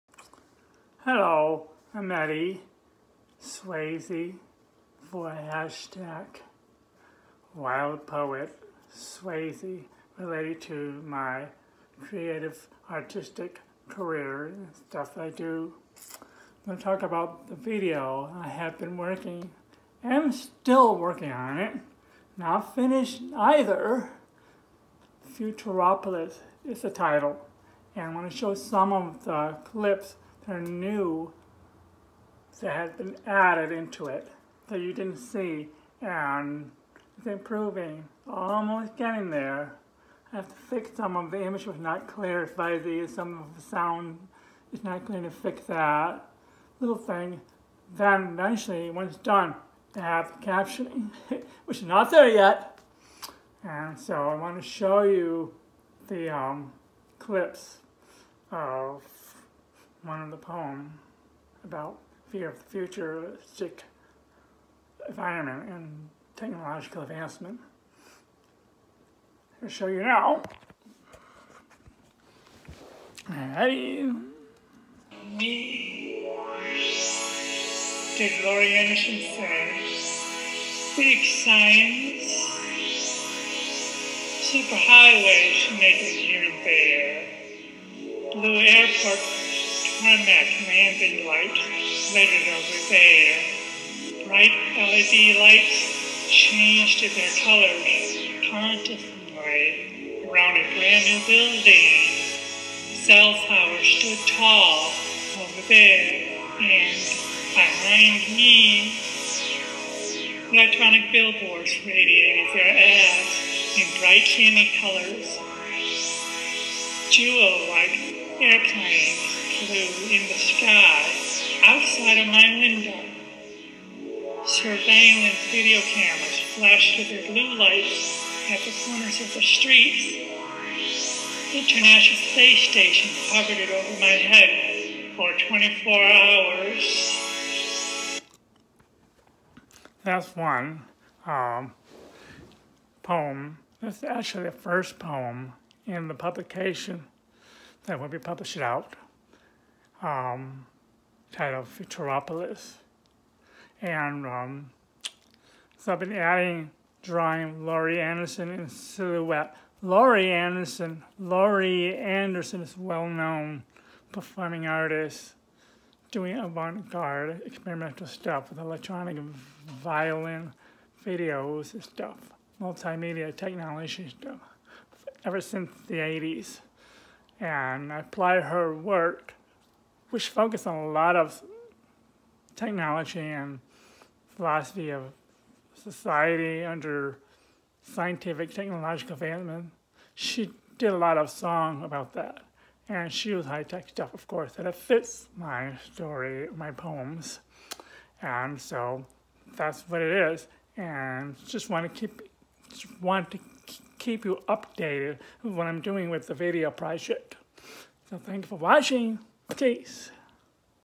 Show Clips of Videos in Futuropolis Video Project
WildPoetSwayze Here is a video of myself showing some clips of video in my Futuropolis video project. Just updating what I'm doing with the project. Enjoy!